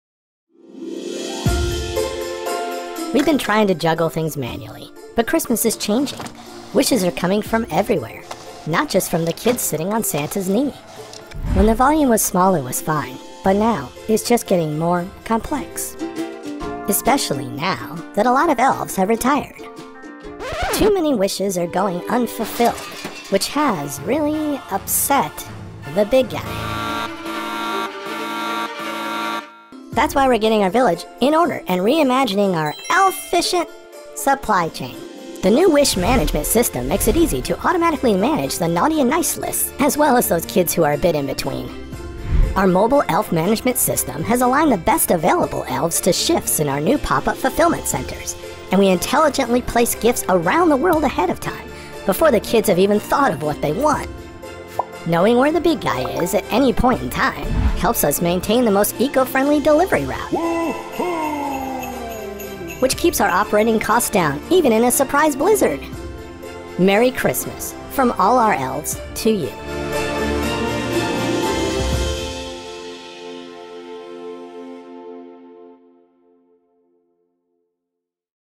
Blue Yonder's Most Elf-ficient Supply Chain 2022
Santa's supply chain goes Blue again this year! Watch how Santa and the elves optimized production and resolved disruption this Christmas.